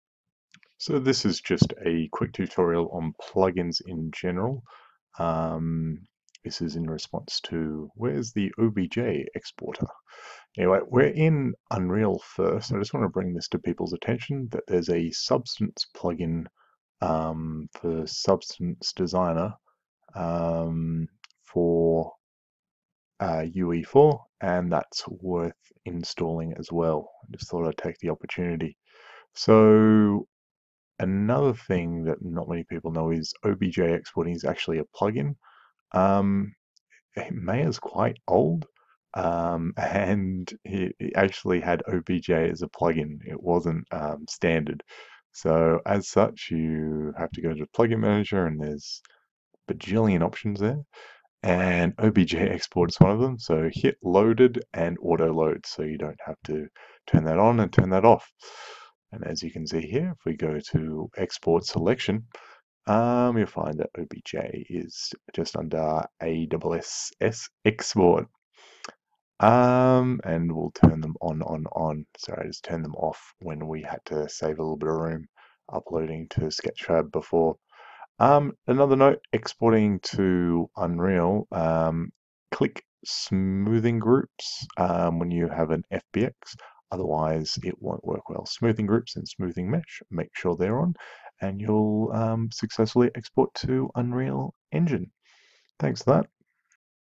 Substance OBJ FBX import export plugins
How to get Substance files working in Unreal Engine
How to get OBJ working in Maya
How to send FBX files to Unreal Engine